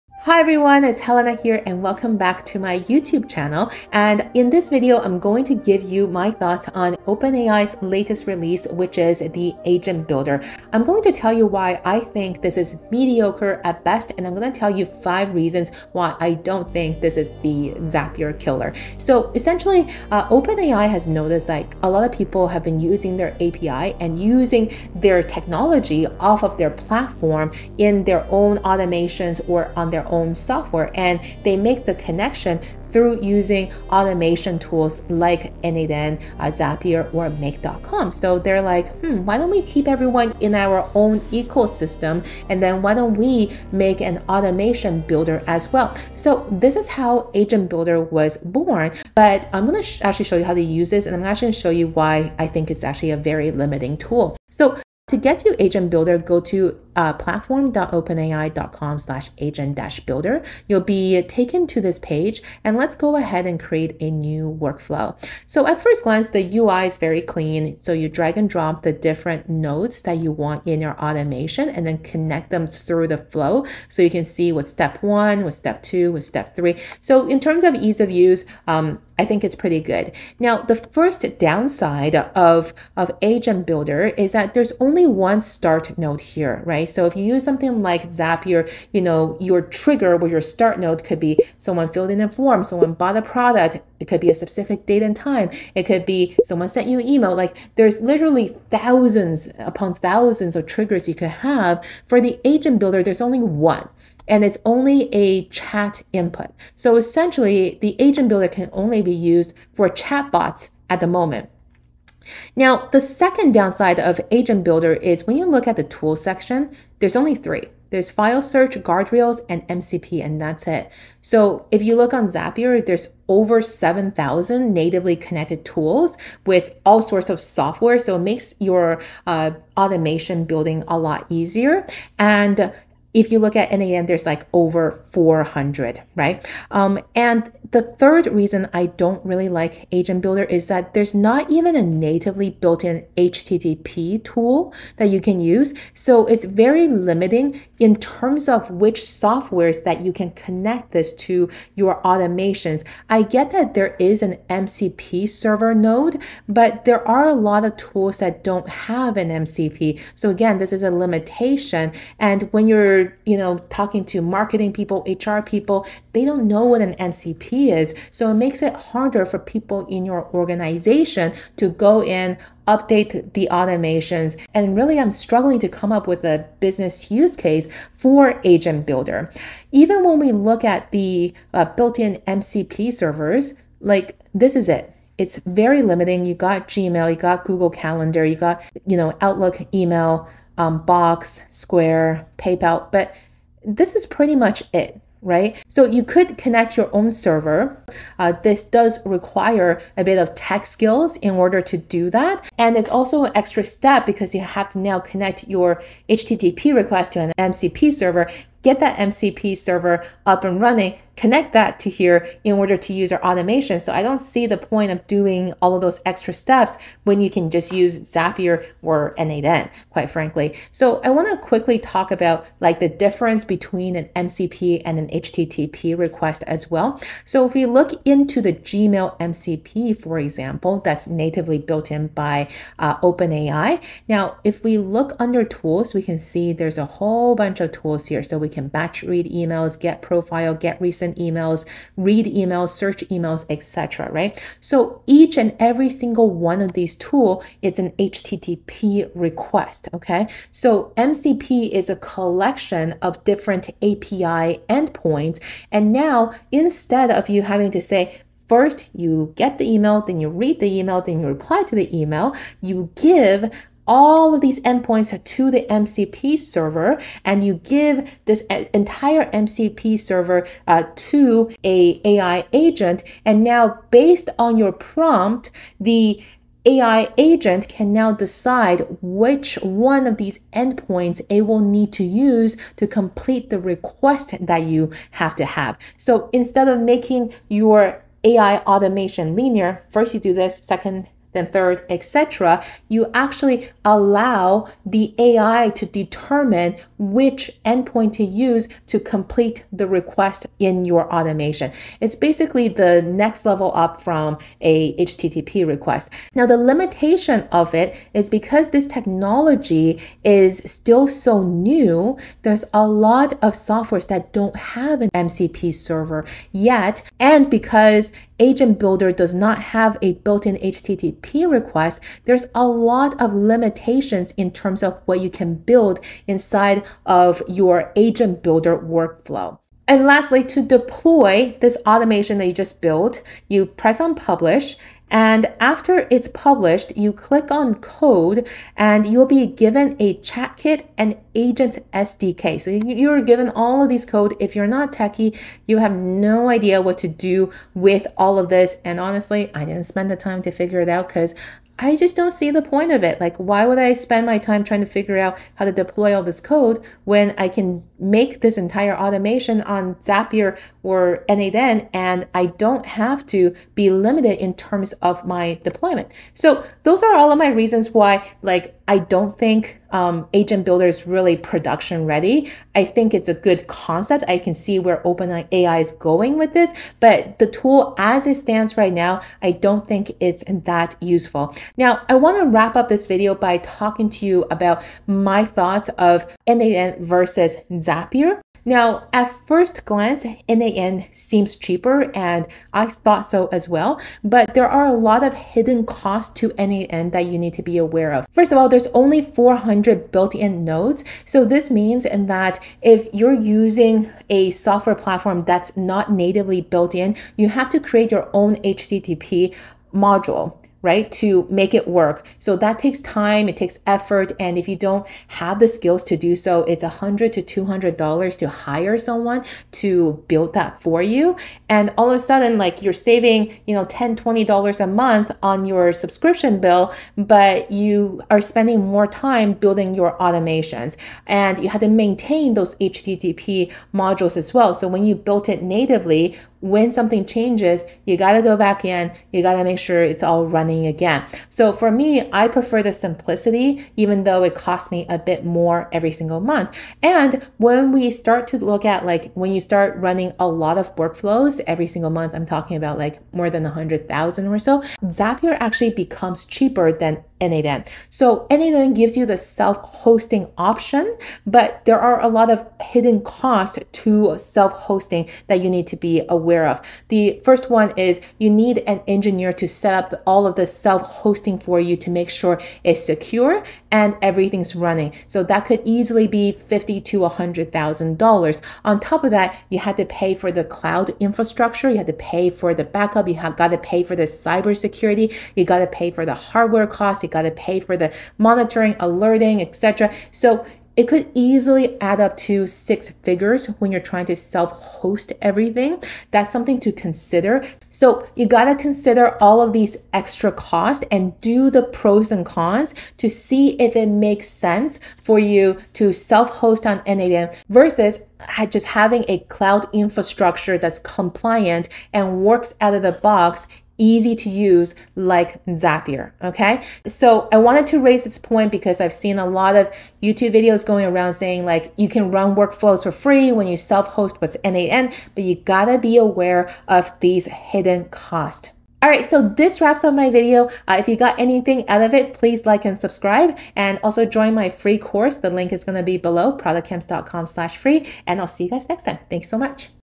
4 Reasons Why OpenAI's Agent Builder is NOT the Zapier Killer
In this video, I share my thoughts on OpenAI's latest release, Agent Builder, and I don't think it lived up to the hype.

There's only 1 trigger that can be used. There are not many natively built in apps to connect to. There's no way to make HTTP requests. There's only 1 method for deployment.

I'm sure they will make it better in the future, but I think it's still got miles to go before it's ready for mass adoption.